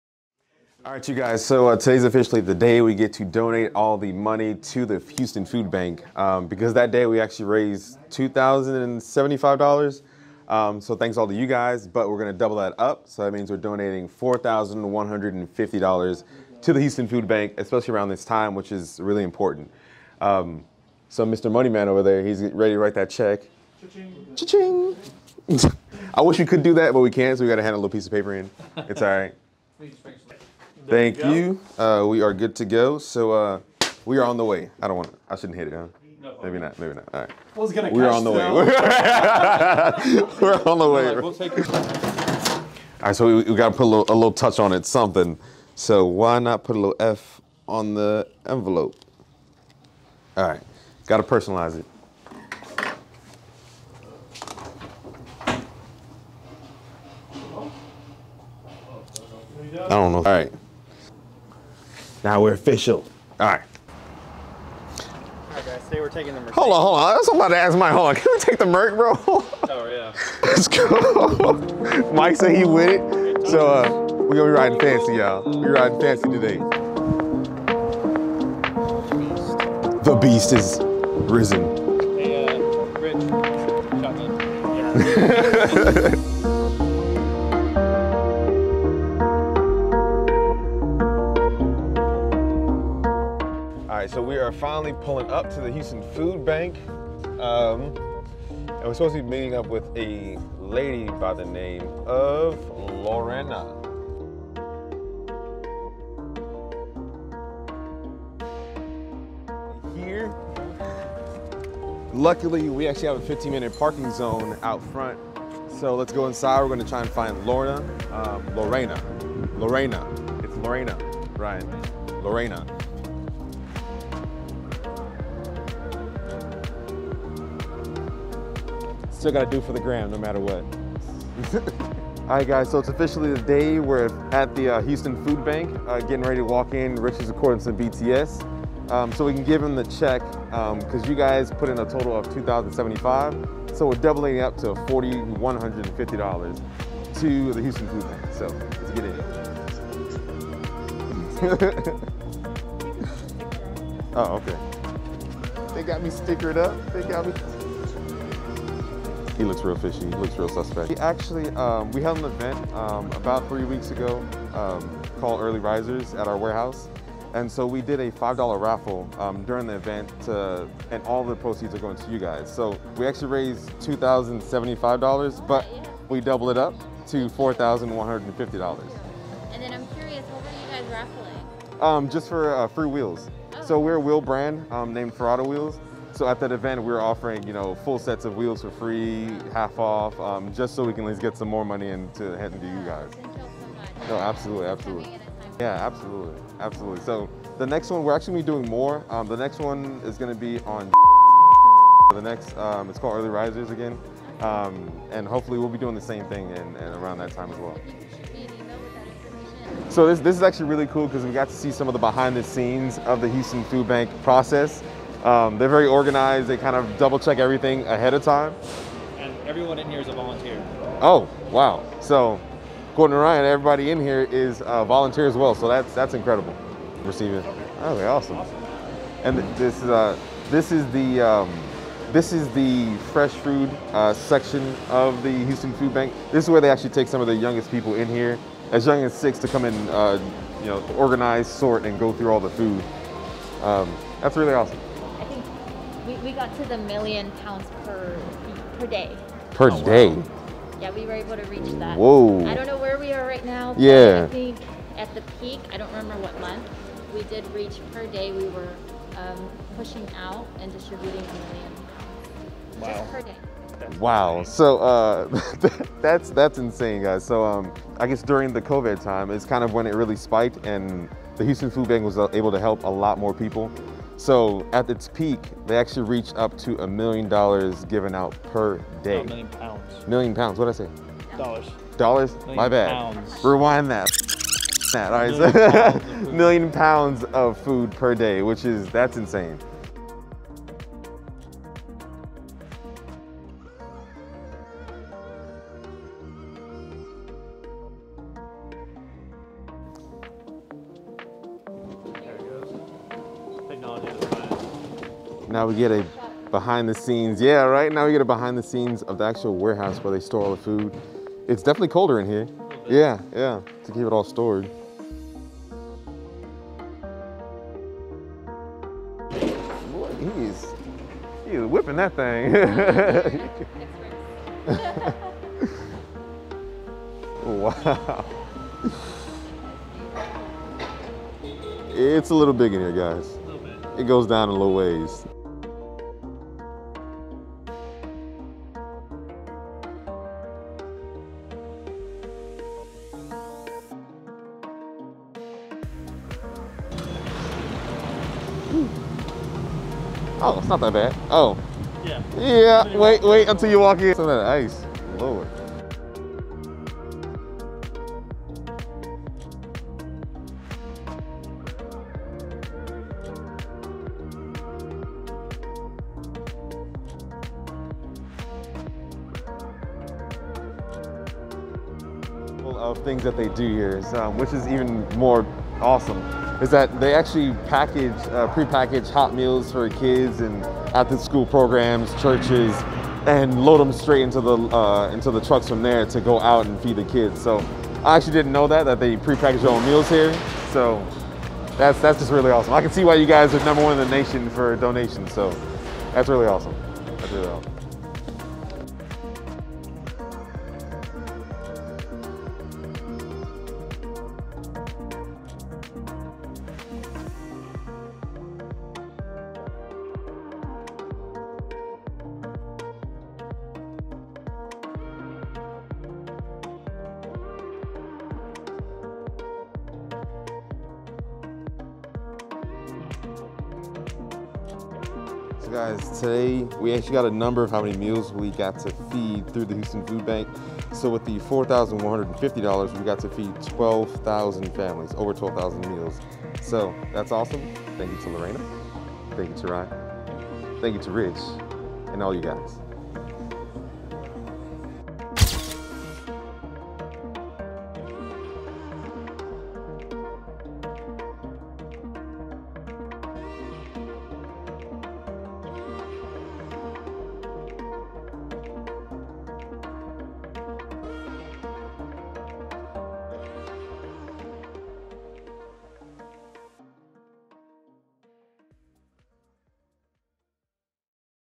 Ferrada Wheels
We had the honor of Donating $4150 to the Houston Food Bank. This was all thanks to those that attended the Early Risers Car Meet and joined the $5 Raffle.

During the Early Risers Car Meet the Raffle raised at total of $2075, but we decided to DOUBLE UP all of the proceeds to do our part in helping out as many families as possible during these hard times.

With the $4150 donated it is able to provide 12,450 meals to those in need.

Thank you again to everyone that participated in the $5 Raffle